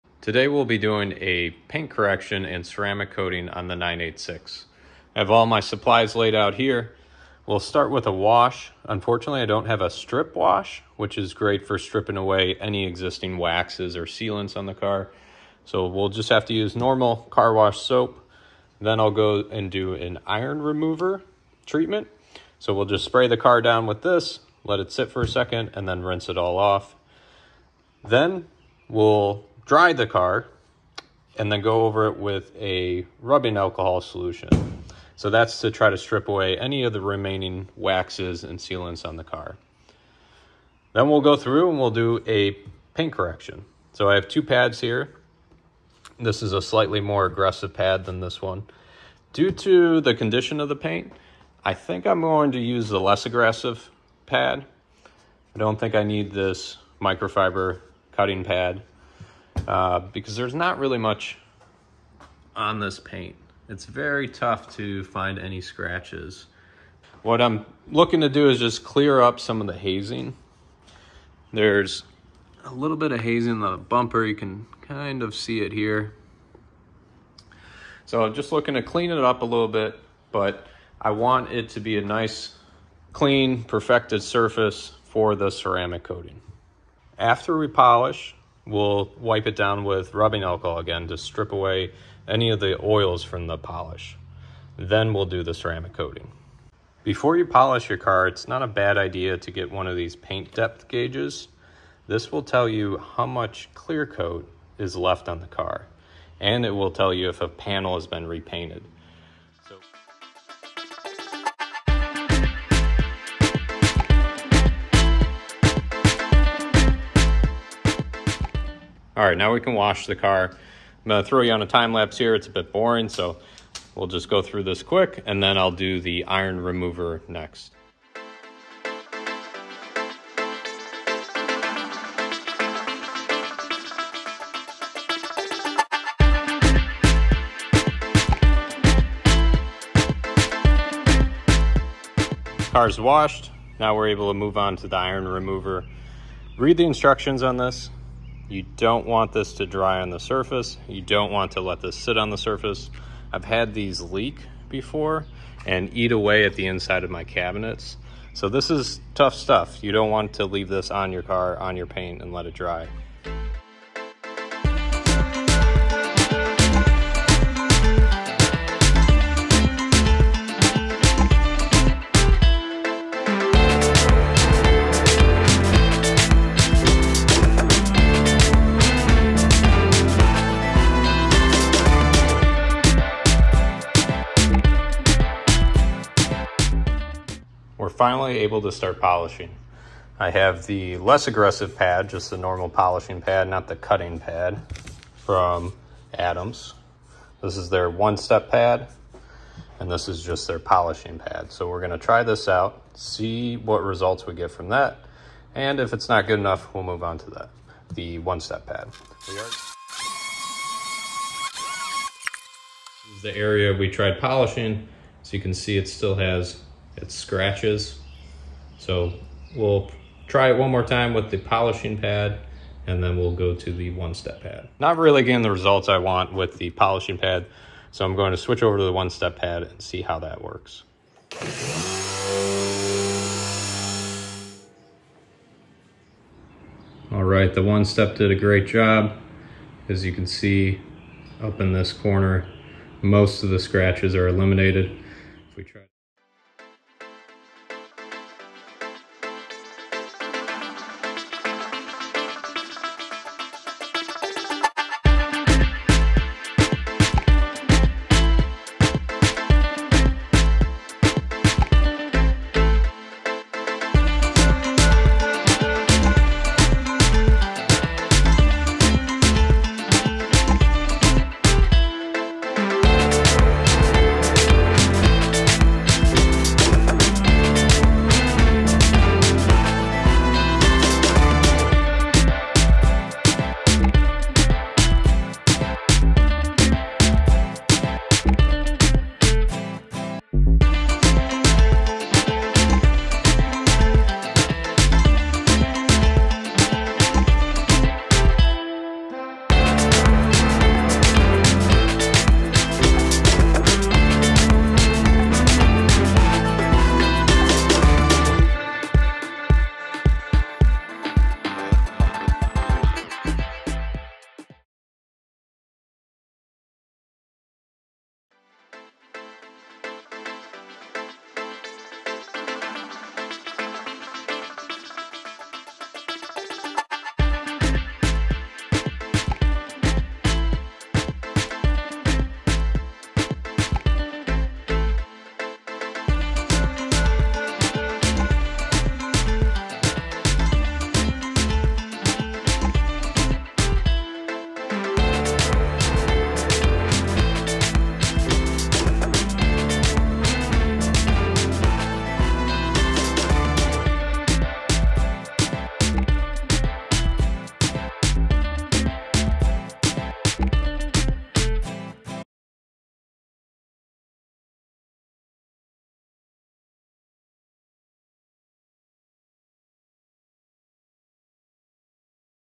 Perfecting the Paint on the 986 Project Car!!! Polish and Ceramic Coating Boxster S 550 Spyder
Polishing and ceramic coating the 986 project car. The paint was already in great shape, but now it is nearly perfect! I did a one step paint correction to remove some minor scratches, then followed up with Adams Graphene ceramic coating.

Supplies Used:
Adam's Graphene Ceramic Coating
Suede Applicators
Adam's One Step Pad
Adam's One Step Polish
Iron Remover
70% Rubbing Alcohol
Microfiber Towels (that won't scratch your paint)
Adam's Wash Bucket
Adam's Premium Foam Cannon
Milwaukee Polisher
Milwaukee Leaf Blower
Stubby Milwaukee Leaf Blower Extension
Giraffe Tools Pressure Washer (Love this thing!!!)